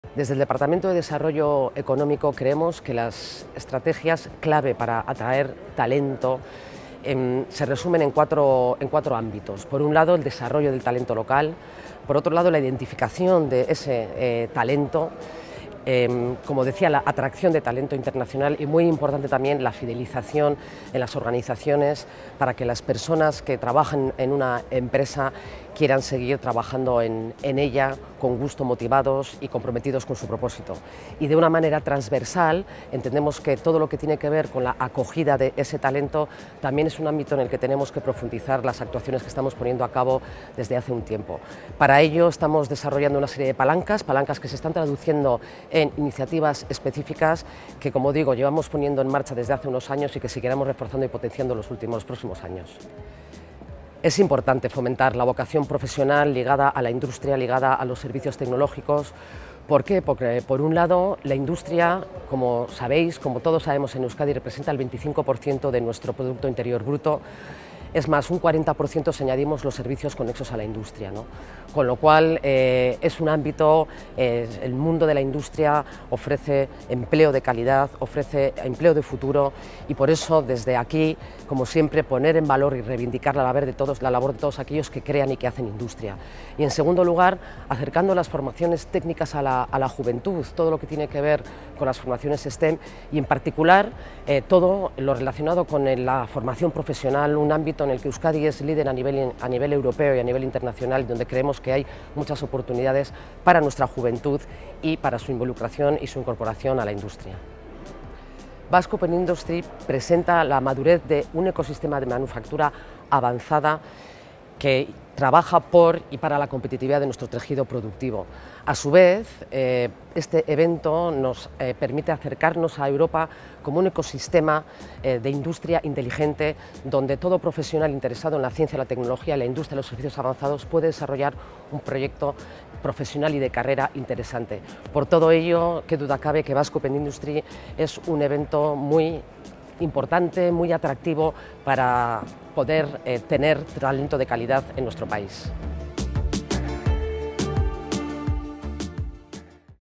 LEYRE MADARIAGA: "El sector de la Industria Vasca ofrece empleo de calidad y de futuro"
En el pasado Basque Open Industry, Leyre Madariaga, directora de Transformación Digital y Emprendimiento del Gobierno Vasco, nos habló del talento. Considera que, para fomentar la vocación profesional industrial, hay que poner en valor a la Industria Vasca. De hecho, "la industria representa el 25% del producto interior bruto; el 40% si tenemos en cuenta los Servicios Avanzados. Es un sector que ofrece empleo de calidad y futuro en Euskadi. "

Las jornadas Basque Open Industry, celebradas el pasado 13 y 14 de noviembre, suponen un punto de encuentro para la Industria Inteligente vasca. La cita contó con más de 2200 inscritos y la participación de 158 empresas en un evento que contó con una exposición de 15.000 m2 y presencia de 24 países de todo el mundo. Así mismo, se trataron temas como la transición digital y medioambiental, los retos demográficos, la internacionalización de la industria vasca, la innovación y nuevas tecnologías de gran potencial como la Inteligencia Artificial.